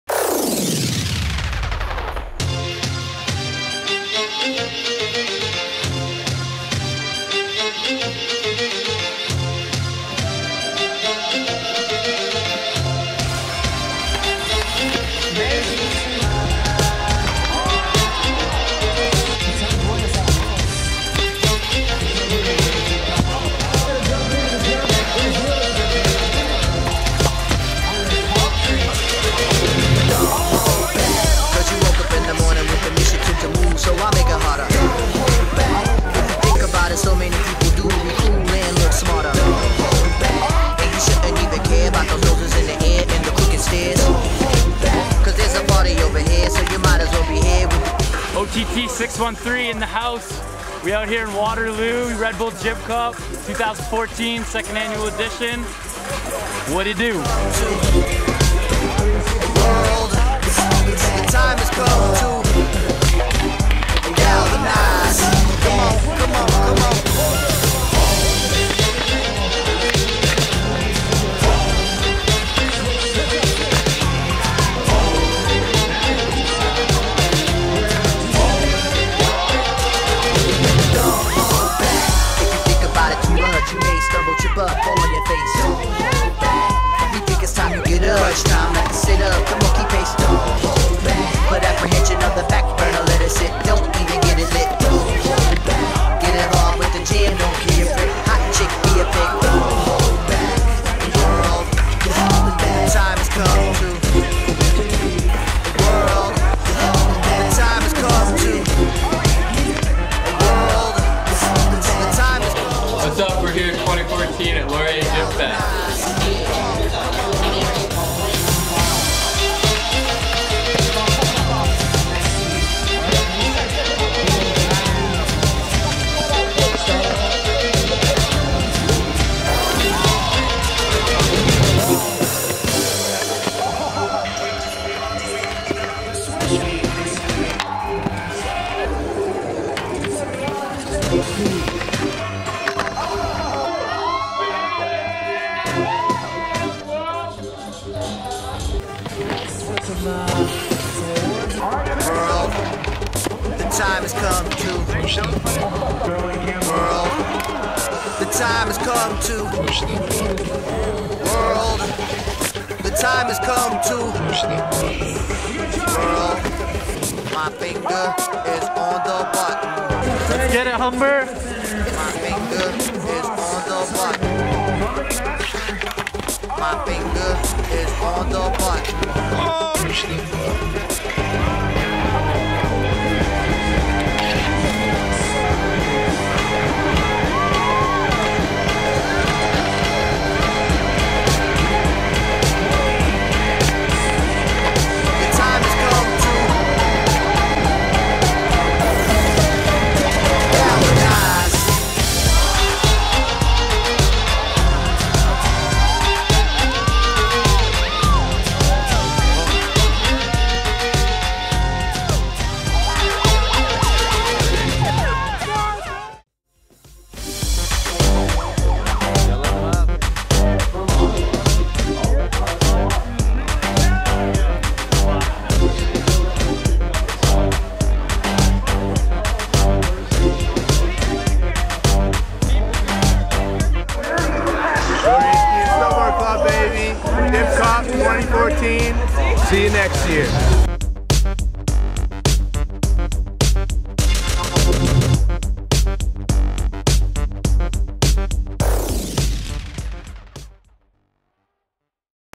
2014 Red Bull Jib Cup Men's Ski
The Official 2014 Red Bull Jib Cup recap video.
Hosted by Laurier University
What was a cold day laid host to a heater of an event hosted by Red Bull.
Teams from Guelph U, Ottawa U, McMaster U, Carleton U, Humber and Laurier came to the Laurier University campus for the rail jam.

Overall Winners:
Carleton University
Steve Arnburg
Konrad Kulikowski
Graham Knox
Josh Harris
Giulia Marcoux
Rachel Penmen

Men's Snowboard:
1. Liam Robinson (Humber)
2. Michael Walker (Guelph)
3. Nick Venditti (Guelph)

Men's ski:
1. Matt Dewar (Ottawa)
2. Chris German (Ottawa)
3. Lucas Vianna (Humber)

Woman's Snowboard
1. Emanuelle Boudreaux (Ottawa)
2. Elise Paddy (Guelph)
3. Giulia Marcoux (Carleton)